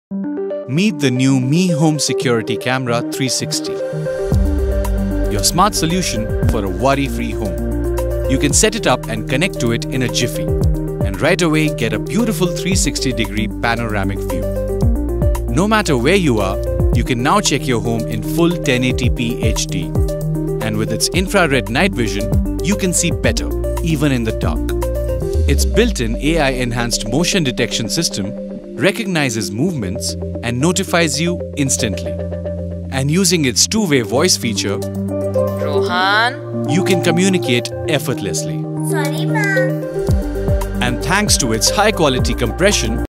Camera Securitate Xiaomi 360 disponibila pe Geekmall.ro
Camera Securitate Xiaomi 360
- imagine FullHD 1080p
- camp vizual 360 grade
- imagine de noapte cu infrarosu
- detectare AI a miscarii
- Talkback
- codificare H.265
- instalare inversata